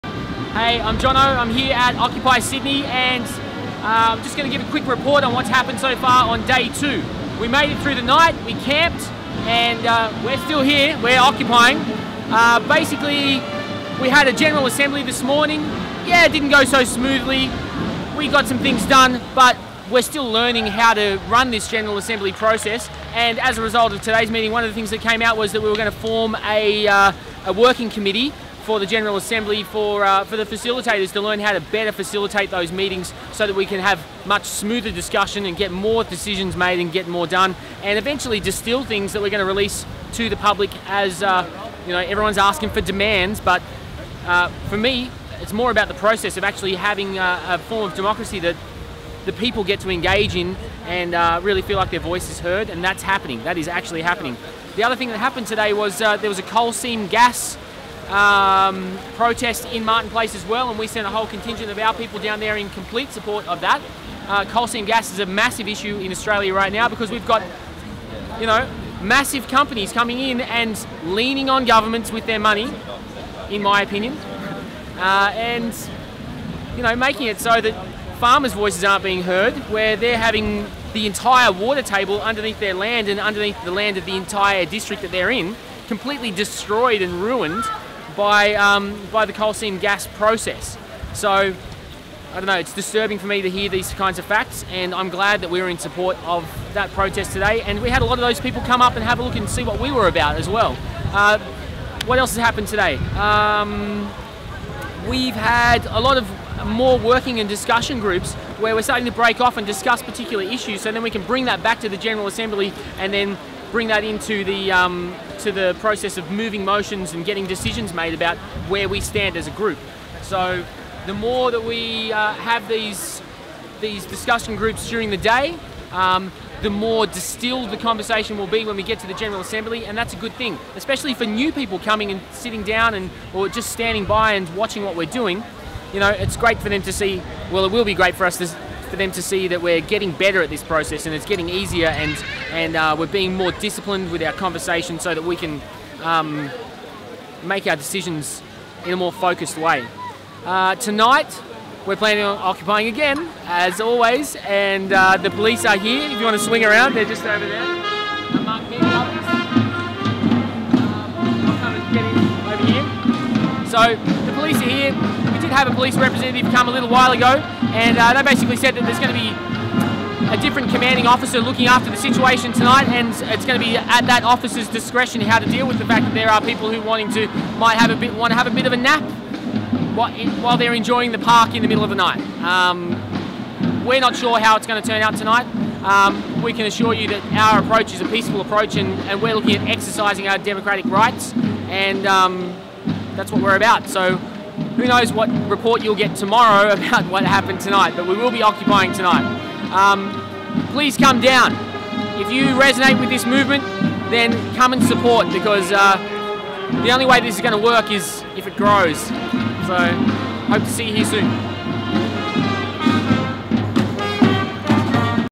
Occupy Sydney: Day 2 Afternoon Recap
Jono gives a brief recap of the second day of Occupy Sydney.